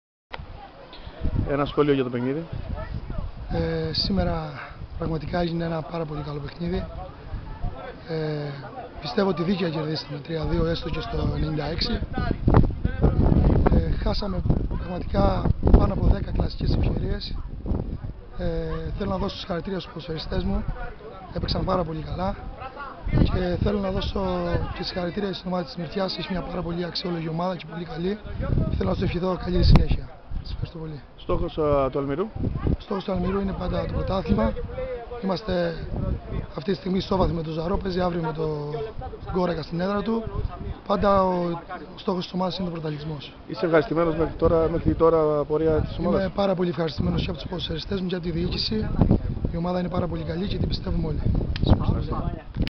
O Βασίλης Μιχελουδάκης στο prismaport.gr
Ο προπονητής του Αλμυρού μίλησε στην WEB-TV του prismasport.gr.